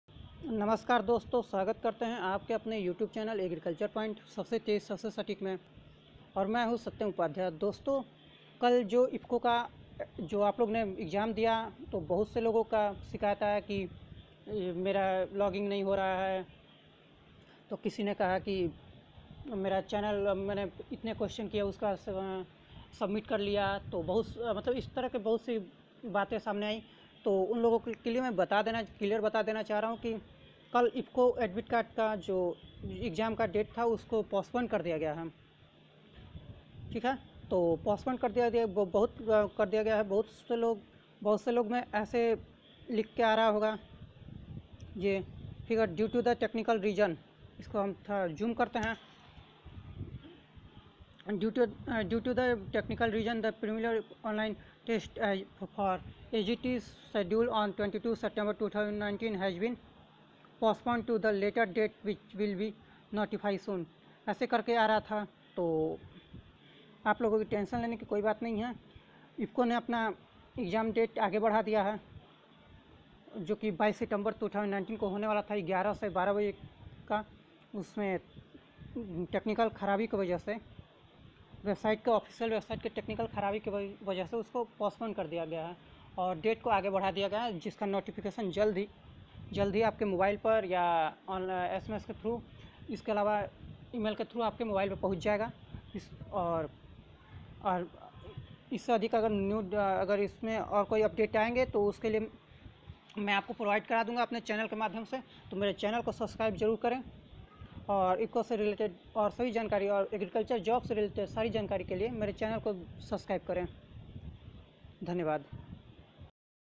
AgricultureGraduateTrainee. IFFCO AGT Exam Postponed😏😏😏
IFFCO AGT Exam Postponed, Check IFFCO Agriculture Graduate Trainee Mock Details Here.